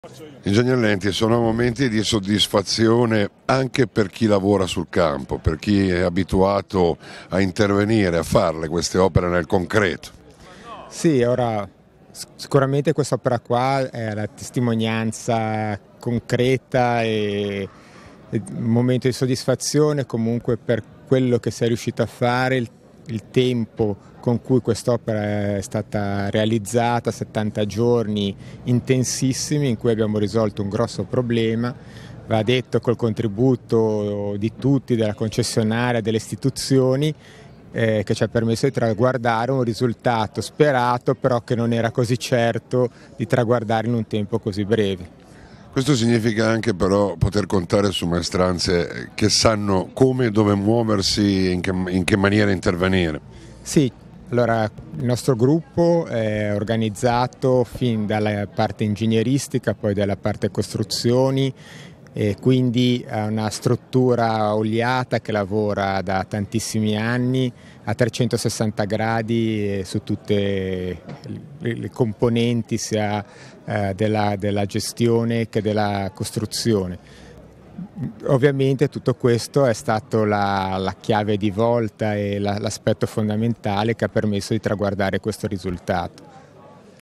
LENTI, DIRETTORE DI TRONCO DI AUTOFIORI: «ATTENZIONE E INTERVENTI SU TUTTA LA TRATTA»
L’ingegner Enrico lenti, direttore di Tronco dell’Autostrada Savona – Torino ha sottolineato l’impegno e l’attenzione lungo tutta la tratta.